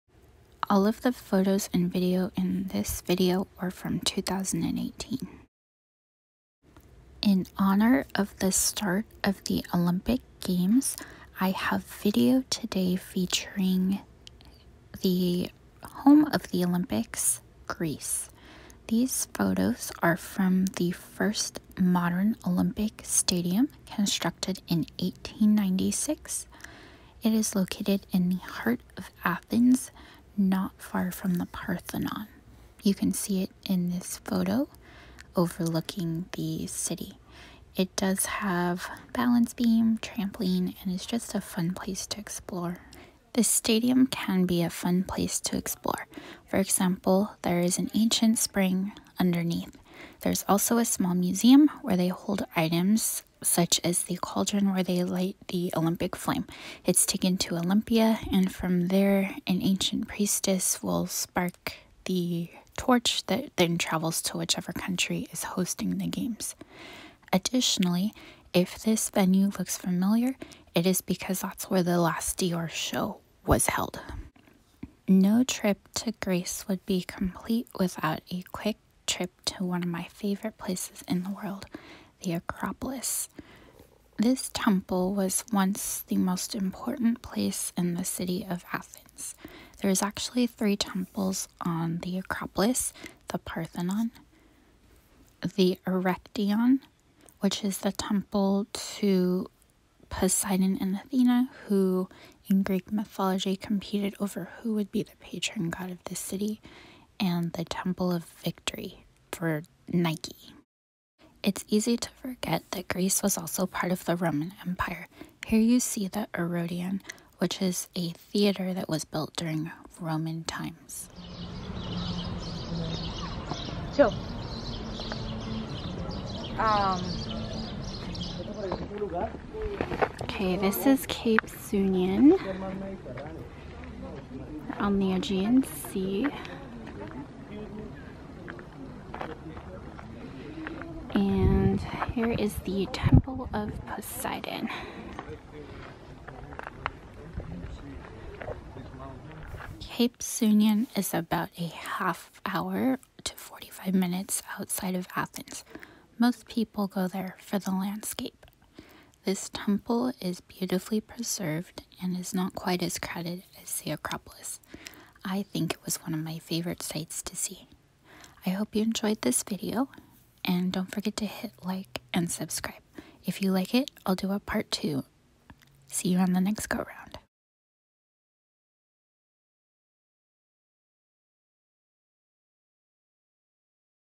Highlights from My Greece Trip (2018) Part One
Hello All,

This video contains content from 2018. In honor of the Olympics, I thought it might be fun to share some of my favorite memories from the home of the Olympics. I hope you enjoy!

Time Stamps

First Modern Olympic Stadium 0:08
Parthenon 1:20
Odeon 1:51
Cape Sunion Temple of Poseidon 2:09
Closing 2:55